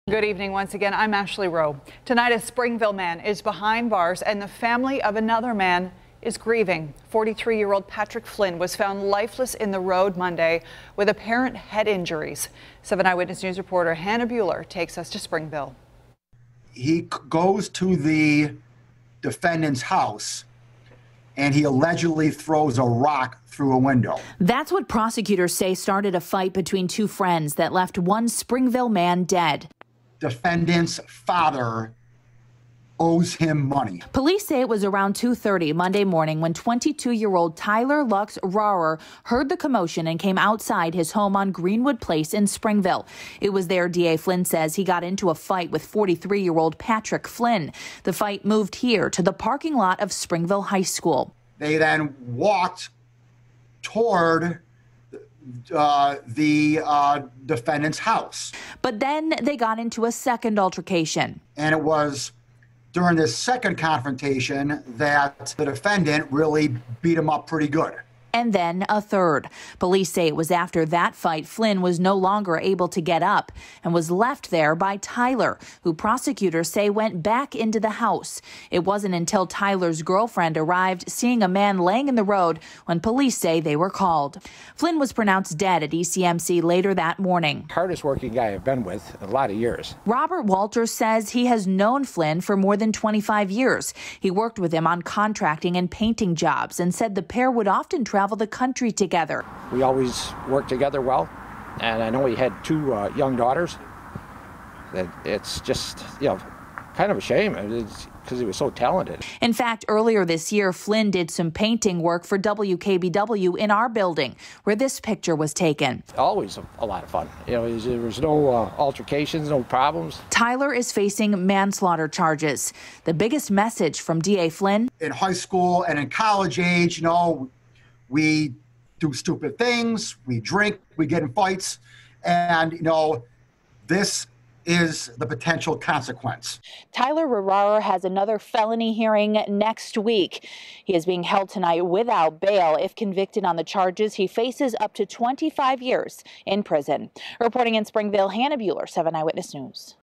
Springville man charged with manslaughter following the death of friend
Springville man charged with manslaughter following the death of friend. Hannah Buehler reports.